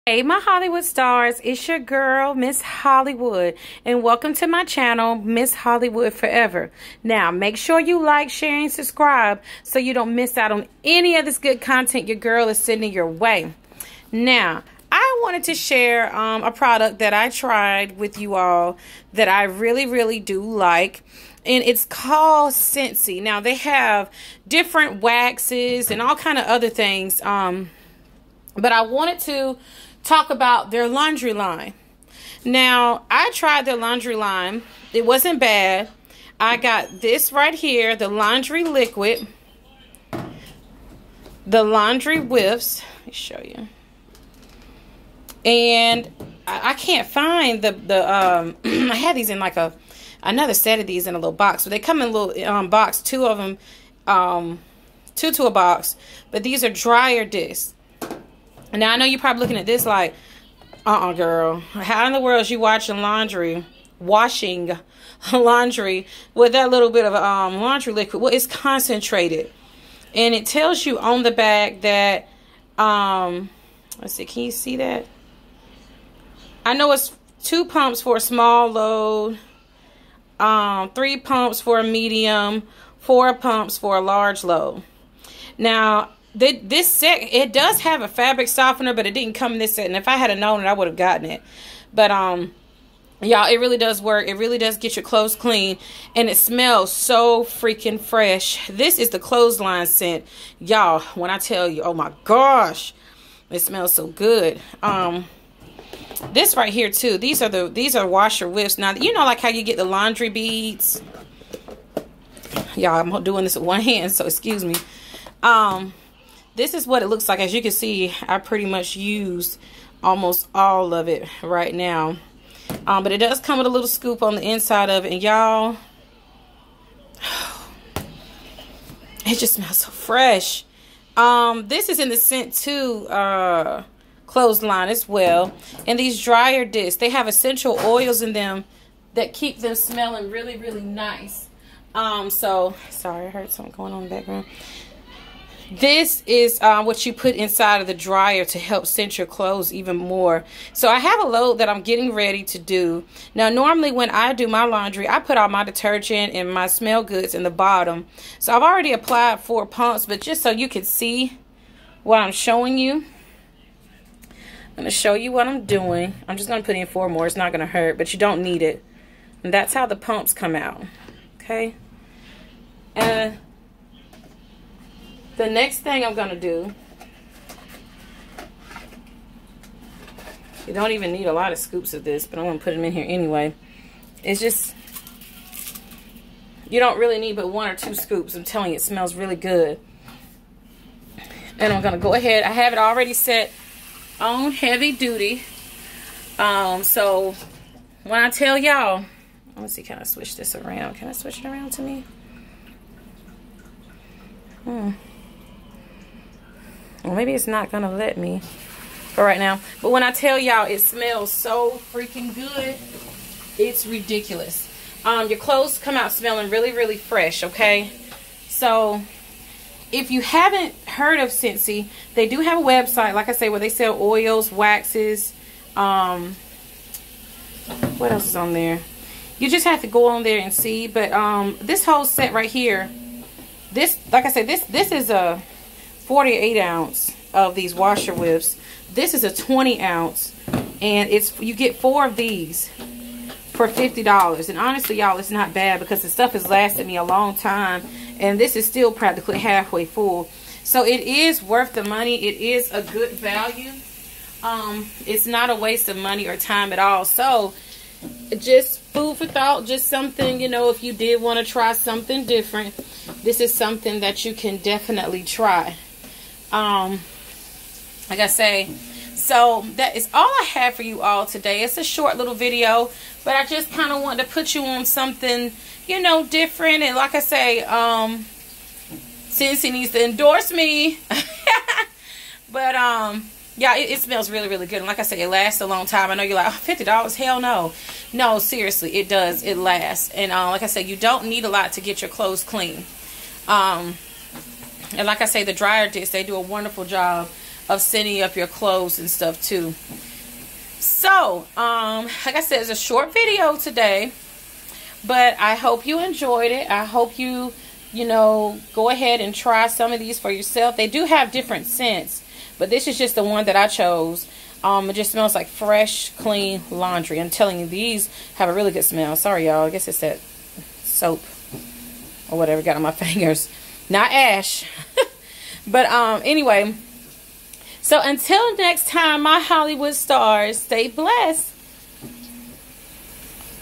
Scentsy Laundry Review
Hey my hollywood stars! I wanted to tell yall about scentsy and their laundry line. Enjoy!